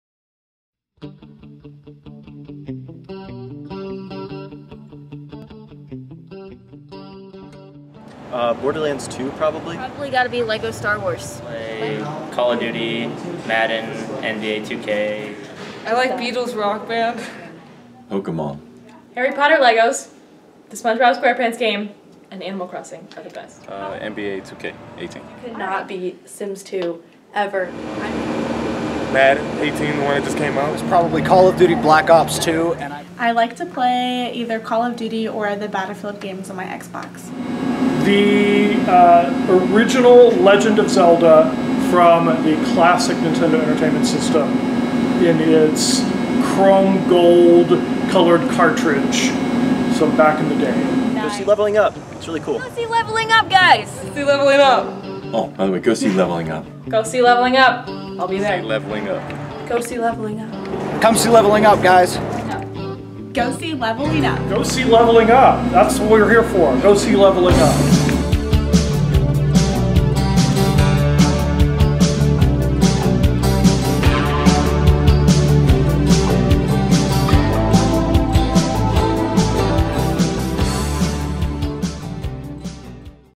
Leveling Up Promo
Leveling Up by Deborah Zoe Laufer
directed by Teresa Durbin-Ames
October 11-14 at 7:30 p.m.
October 15 at 2 p.m.